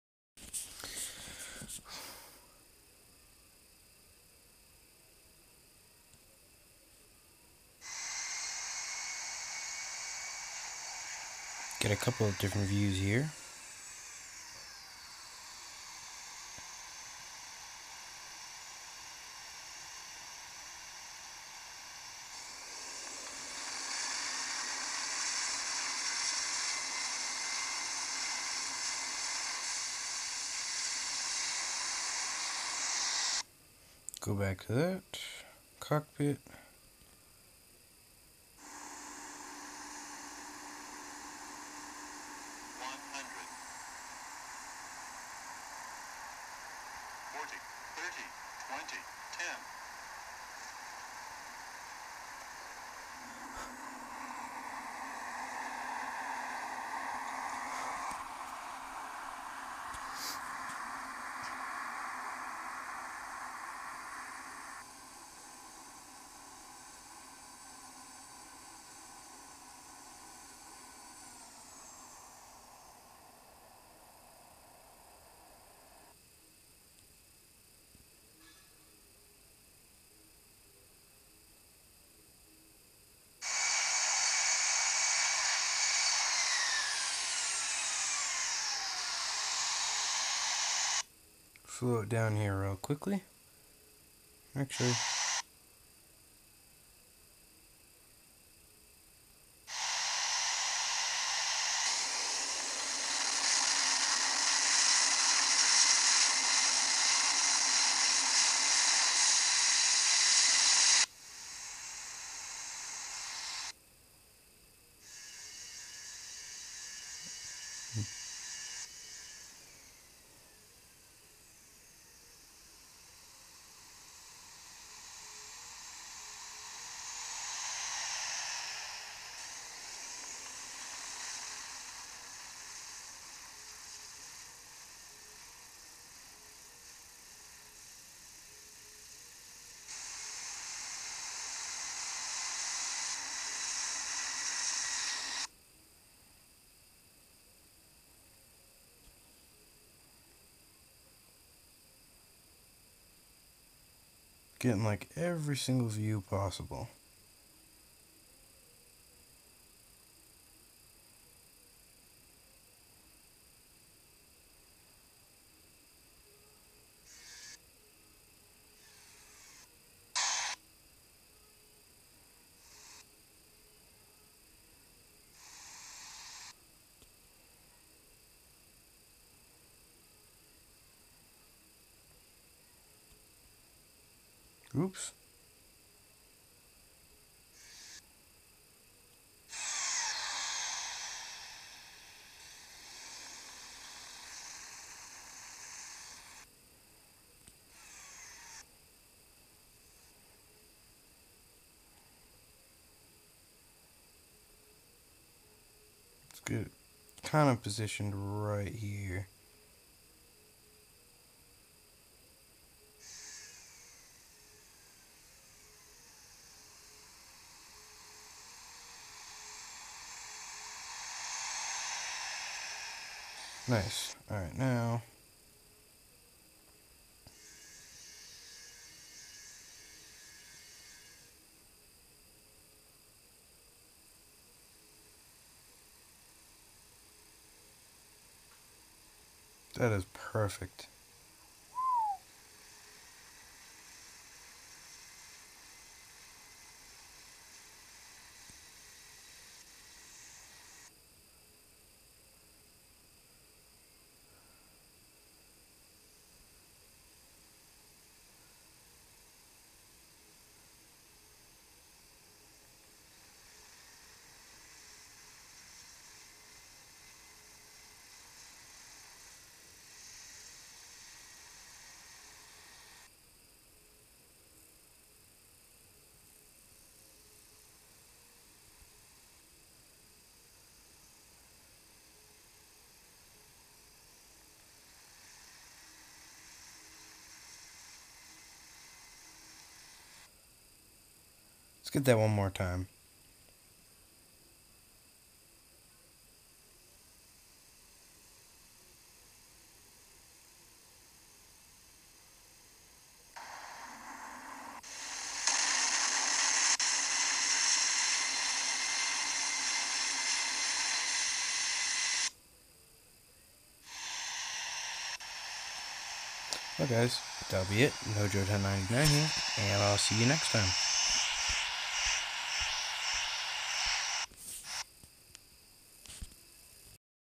Short final | IFFS | Lufthansa Airbus A380-800 | Hawaii
Probably my smoothest landing. Hope you enjoy!